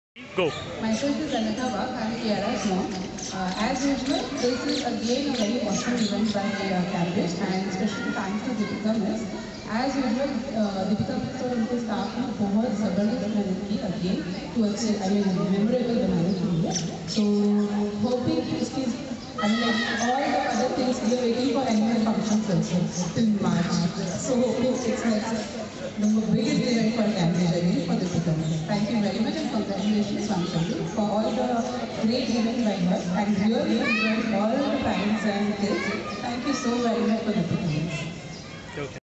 Review by Parents for the Annual Sports day organized by Cambridge Montessori Preschool and Daycare Chembur
Children games
Parents games